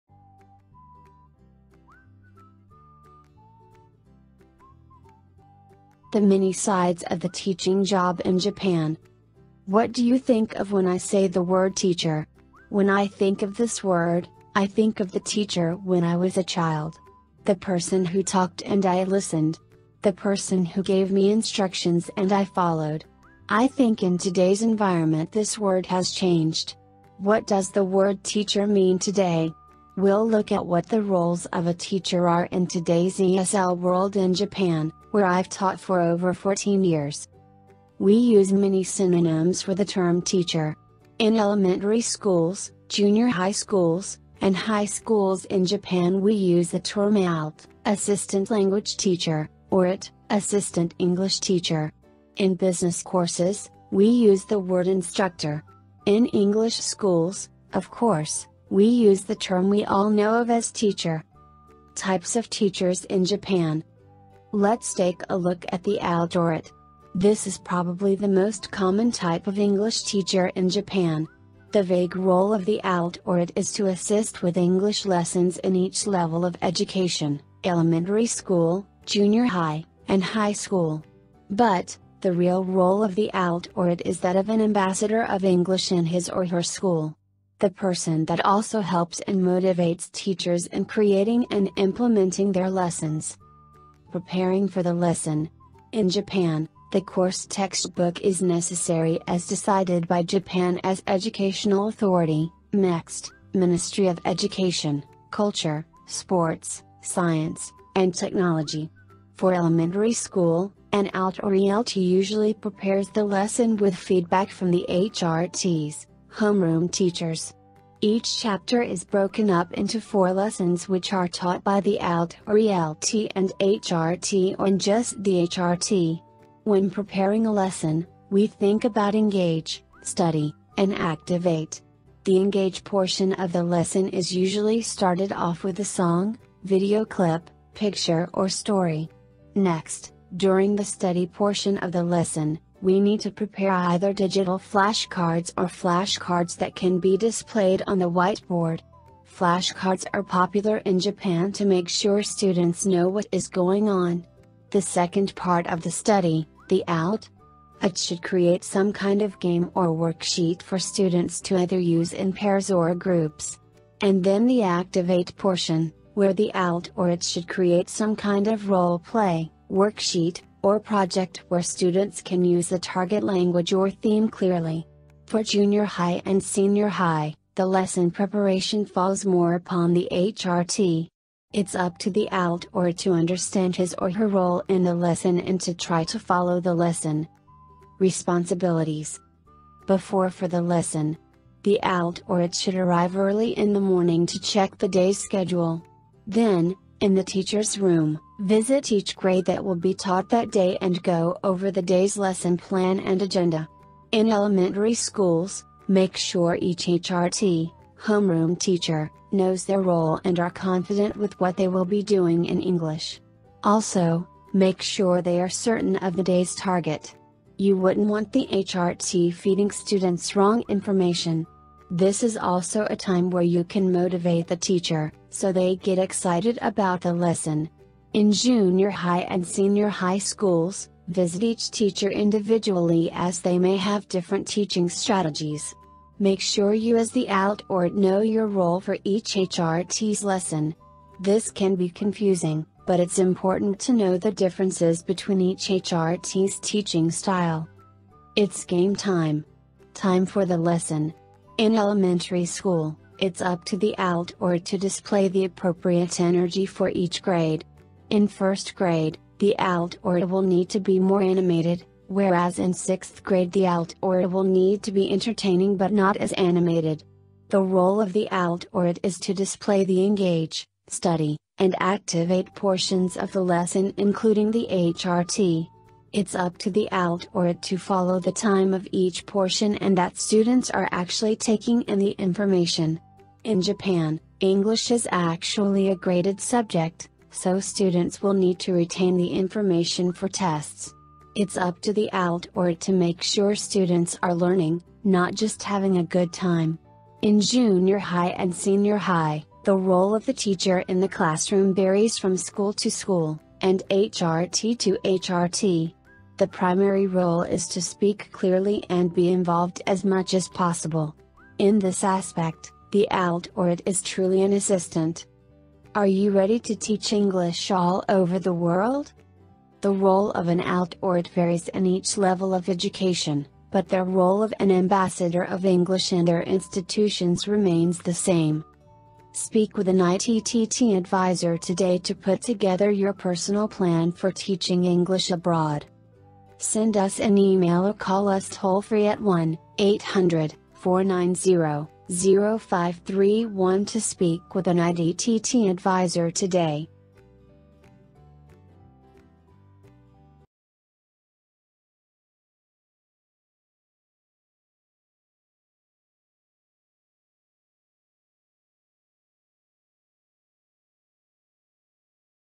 The Many Sides of The Teaching Job in Japan | ITTT TEFL BLOG
What do you think of when I say the word “teacher”? When I think of this word, I think of the “teacher when I was a child. The person who talked and I listened. The person who gave me instructions and I followed. I think in today’s environment this word has changed. What does the word “teacher” mean today? We’ll look at what the roles of a teacher are in today’s ESL world in Japan, where I’ve taught for over 14 years.

We use many synonyms for the term “teacher”. In elementary schools, junior high schools, and high schools in Japan we use the term “ALT” (assistant language teacher) or “AET” (assistant English teacher). In business courses, we use the word “instructor”. In English schools, of course, we use the term we all know of as “teacher”.